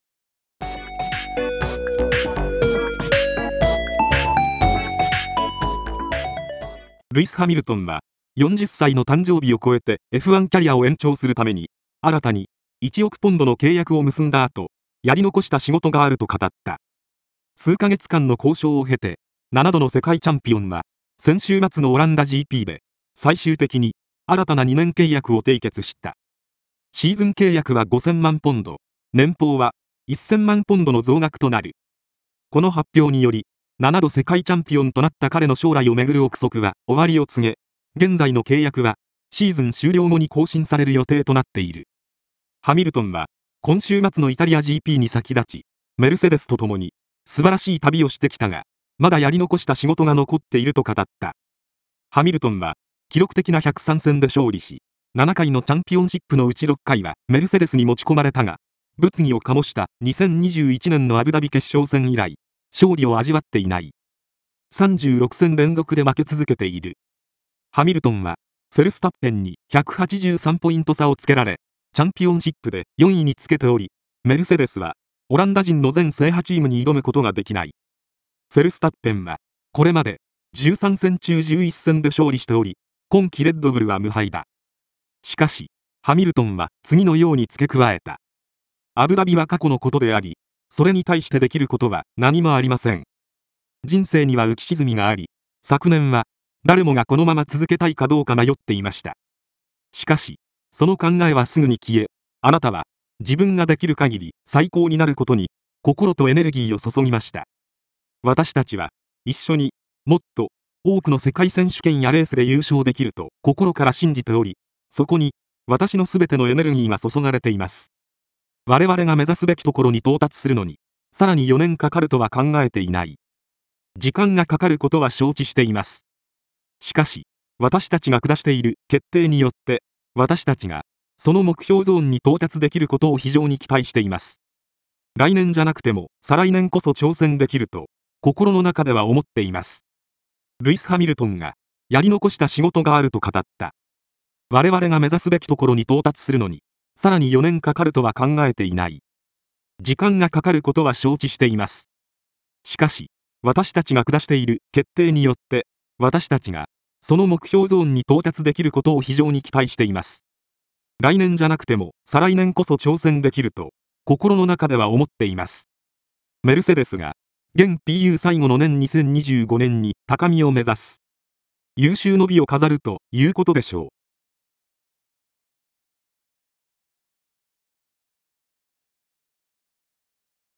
Ｆ1 ハミルトン、メルセデスと新たな契約を結んだ理由：「やり残した仕事」 '23 09 01
Ｆ1情報　ハミルトン、メルセデスと新たな契約を結んだ理由として「やり残した仕事」　とコメント。